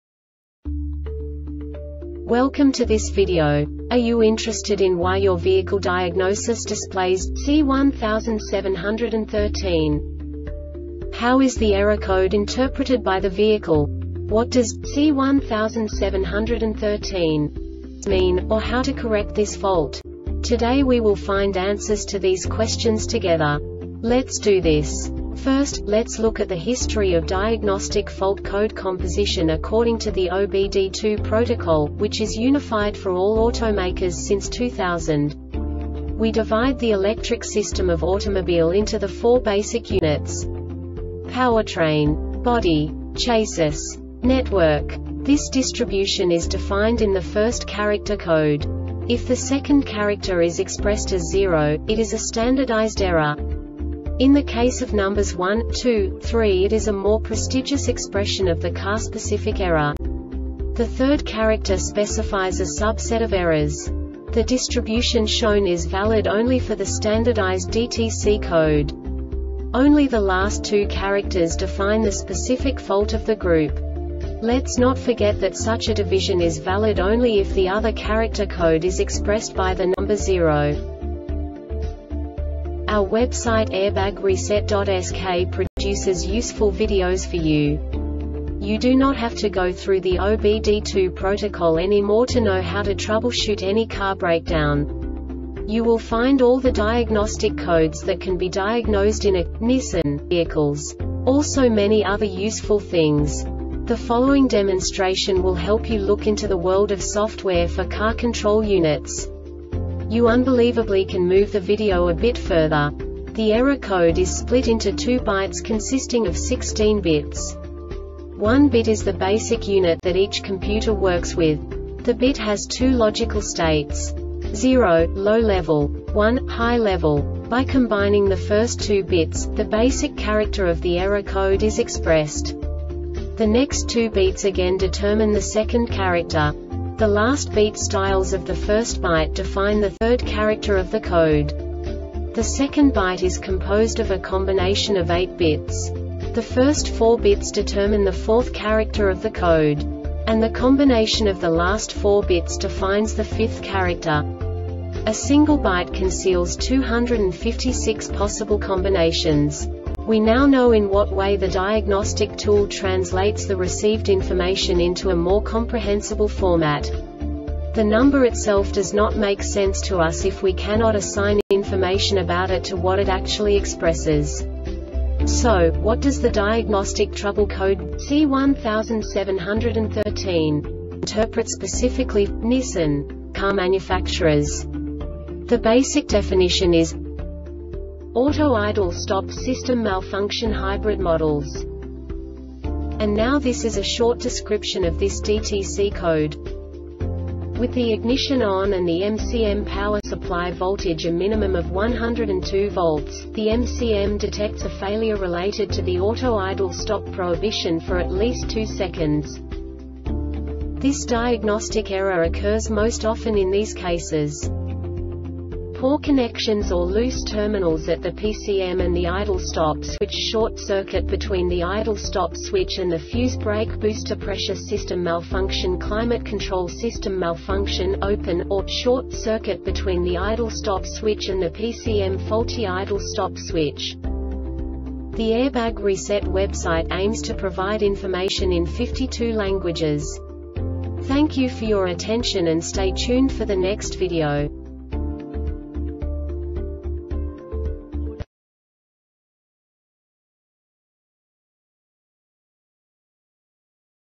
DTC Nissan C1713 Short Explanation
Contents:
0:21 Basic DTC analysis according to OBD2 protocol standard.
1:48 Insight into programming
2:58 A diagnostically understandable description of the Diagnostic Trouble Code
3:05 Basic error definition

Make
Nissan
Code
C1713
Definition
Auto Idle Stop System Malfunction HYBRID MODELS
Description
With the ignition ON and the MCM power-supply voltage a minimum of 102 V,  the MCM detects a failure related to the auto idle stop prohibition for at least 2 seconds
Cause
 Poor connections or loose terminals at the PCM and the idle stop switch Short" circuit between the idle stop switch and the fuse Brake booster pressure system malfunction  Climate control system malfunction  "Open" or "Short" circuit between the idle stop switch and the PCM  Faulty idle stop switch